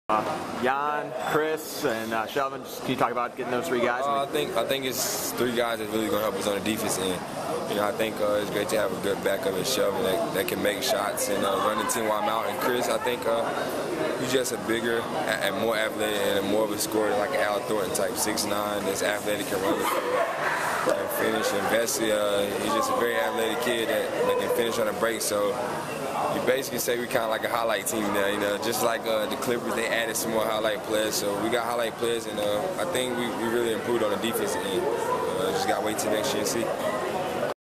John Wall on drafting Jan Vesely and Chris Singleton
John Wall gives his thoughts on his newest team mates.